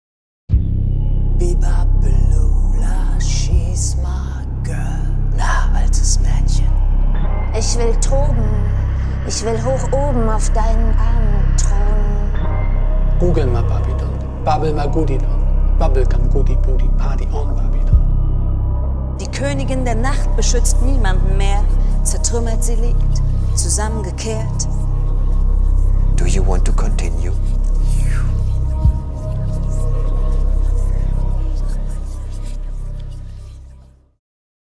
BABYLON KINO TRAILER
Trailer für die Ausstellung
BABYLON - Mythos und Wahrheit
im Pergamon Museum Berlin 2008
Der erste mir bekannte Commercial, in dem Slam Poeten ein Produkt (Ausstellung) bewerben.

Läuft zwischen Juli und Oktober 08 in über 50 Kinos bundesweit.